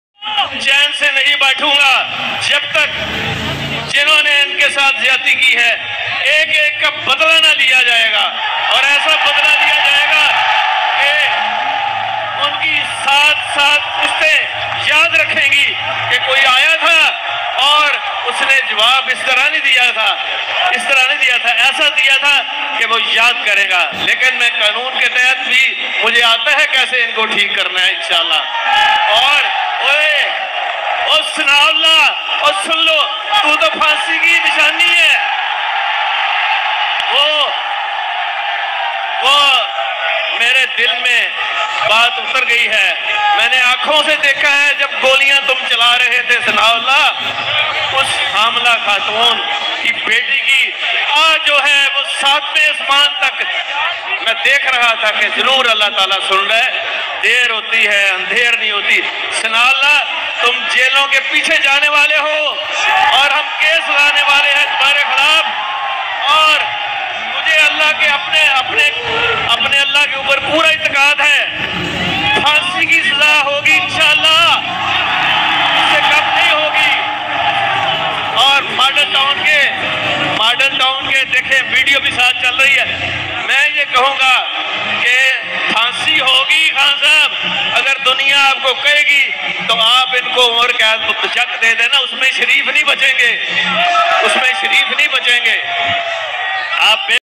"Asa badla lo ga ka tumahri 7 nasly yad rakhy gy" CM Punjab pervaiz elahi got hyper Rana sana ullah.
25 date Imran Khan King attack Rana sanaullah and different pmln (Master bird's and Umais)